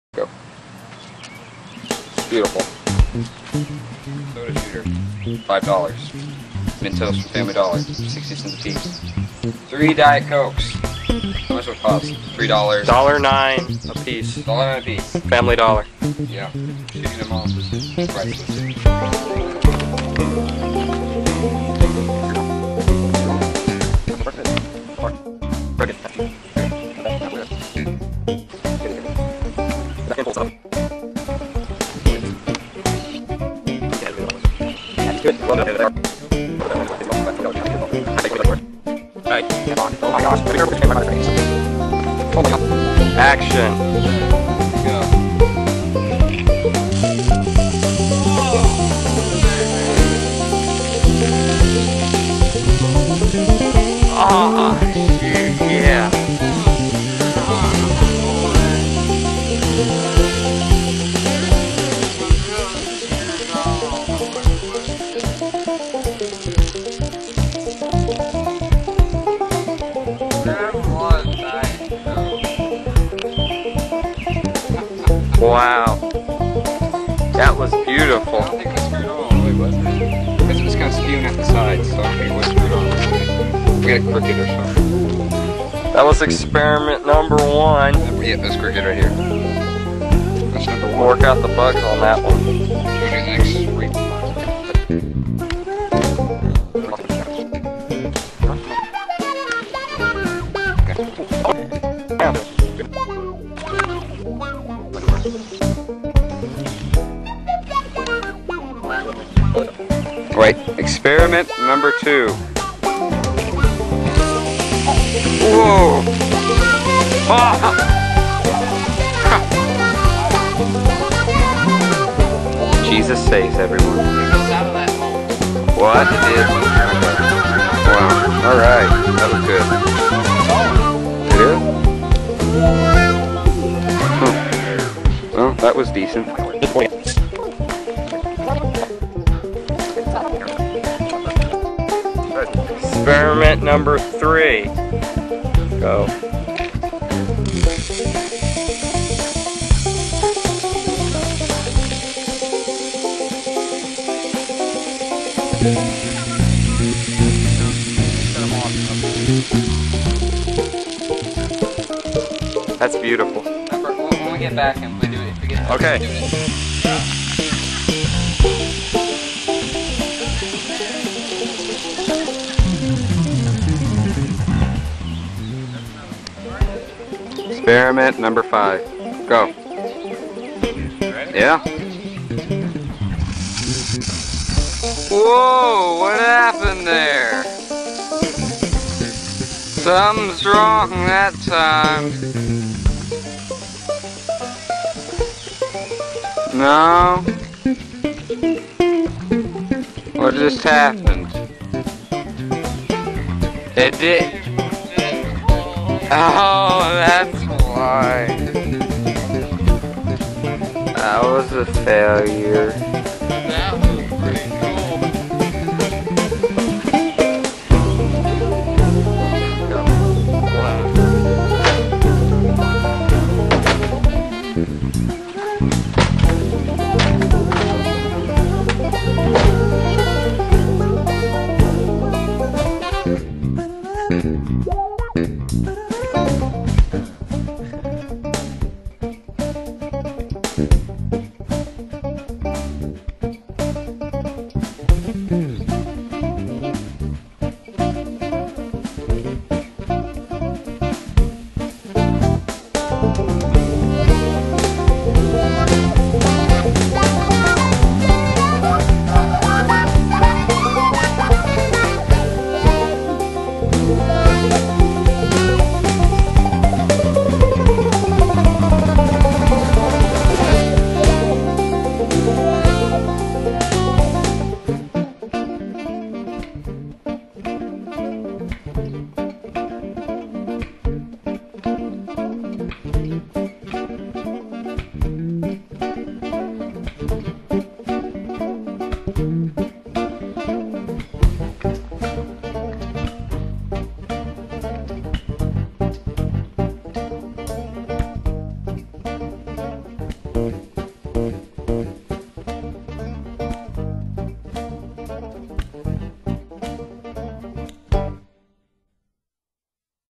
Redneck Nerd's Diet Coke and Mentos Experiment
This is my first video on youtube. It is just another DC and Mentos thing but it will get our foot in the door. Please leave any comments. CuJo and I (maurice) greatly appreciate it. Most of the items used in this video were bought at the local Family Dollar.